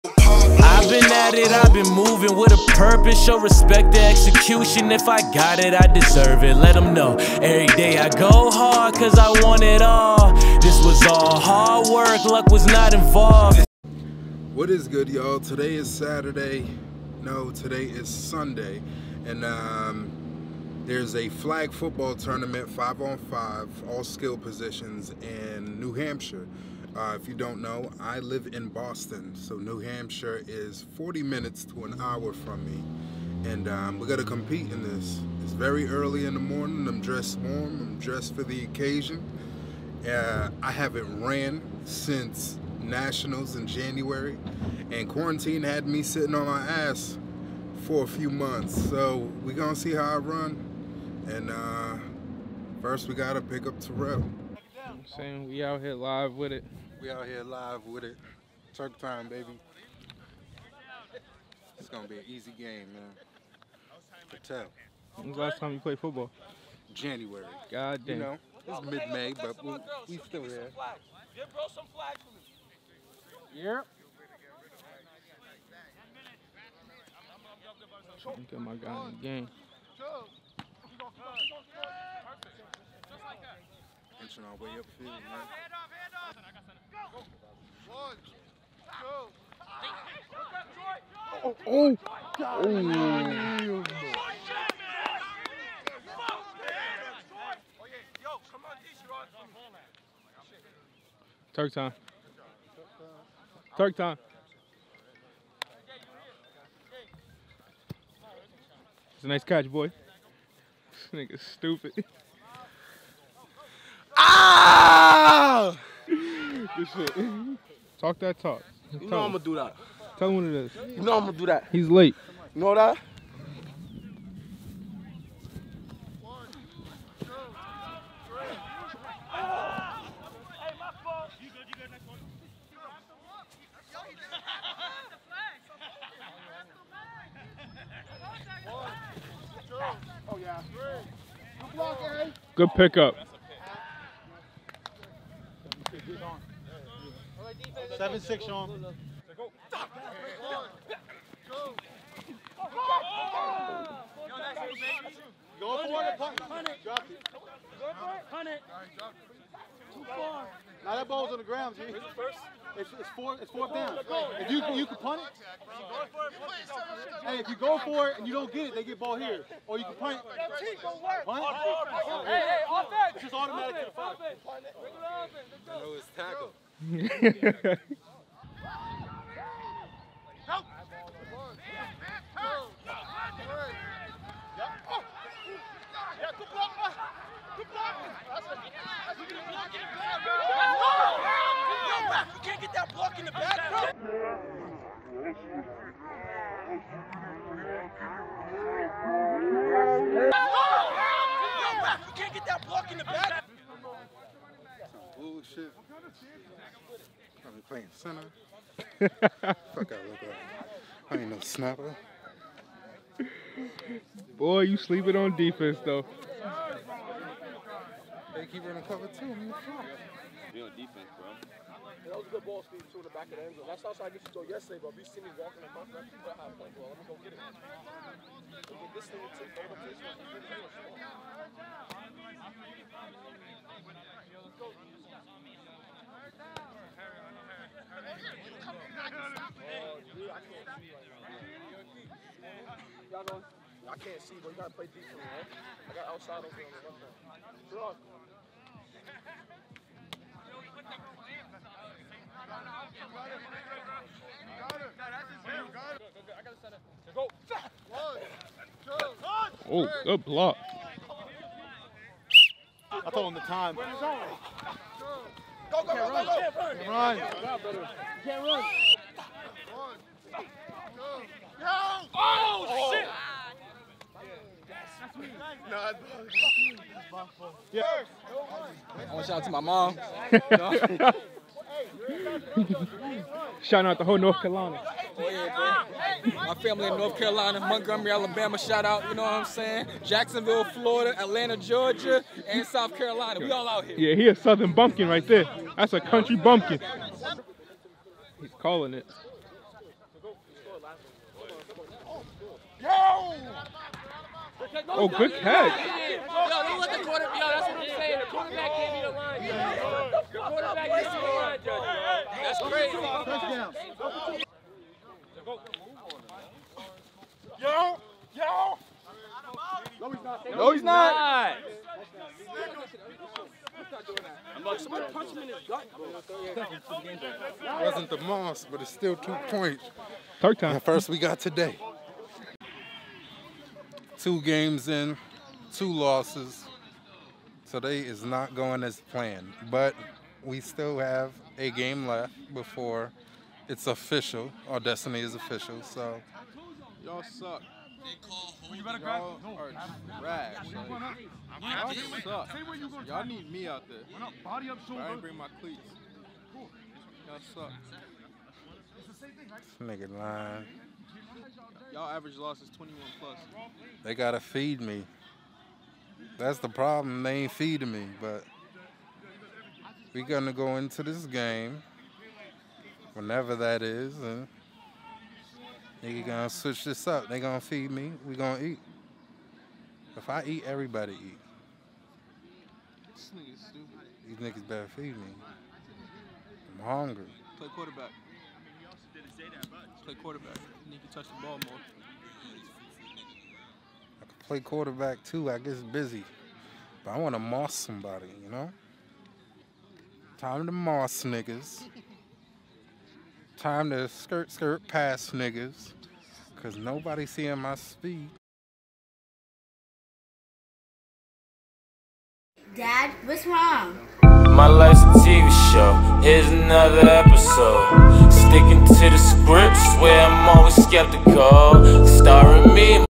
Joined a flag football tournament with semi pro football players . .
🔵TurkTime

👻: chiefblacc